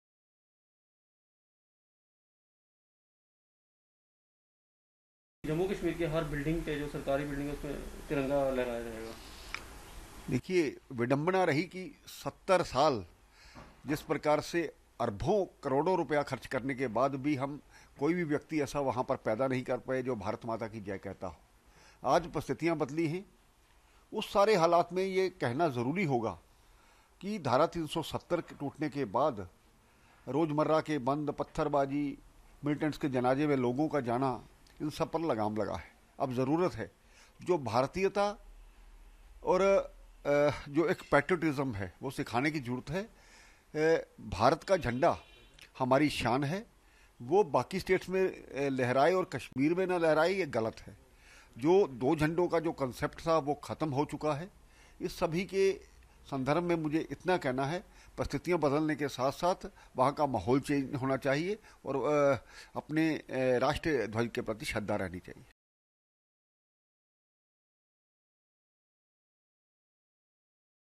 ‘Environment should change’: Kavinder Gupta on tricolour hoisting on J&K’s govt buildings
Jammu (J&K), Mar 30 (ANI): While speaking to ANI in Jammu, the former deputy chief minister of J&K, Kavinder Gupta spoke about hoisting of tricolour in the Union Territory. Gupta said, “It is unfortunate that even we couldn't find a single man there (in Kashmir) who hails Mother India even after spending crores of rupees in the last 70 years. But the situation has changed now.” “After Article 370 repeal, shutdowns and incidents of stone-pelting have stopped. Now we need to teach patriotism. Indian flag is our pride and it is not good if it isn’t hoisted in Kashmir. (SA)